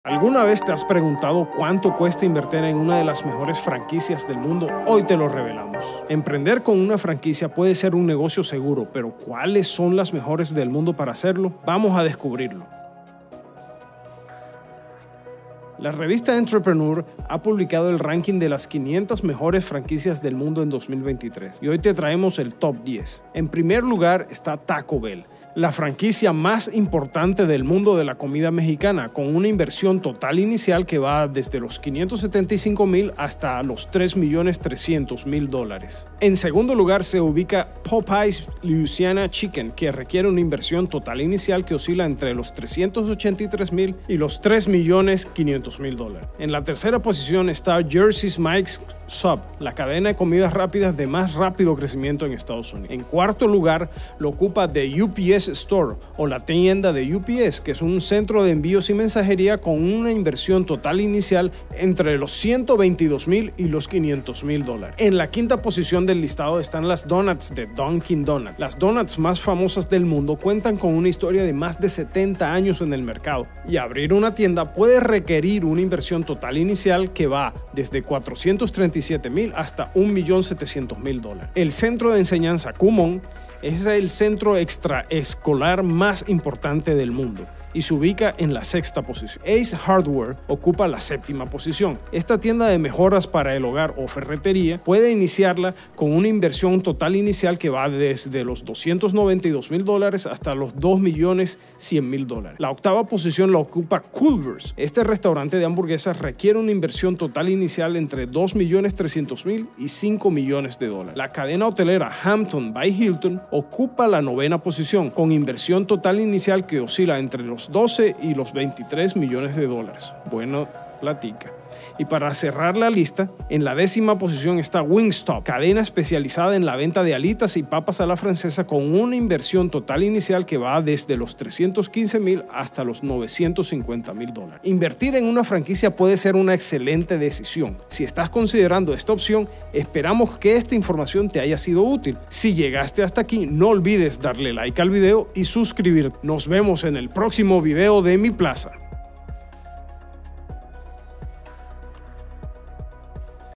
Top 10 Franquicias en el mundo 2024: ¿Cuál es para Ti?
¿Cuáles  son las 10 franquicias más interesantes del mundo en 2024? Aquí te contamos.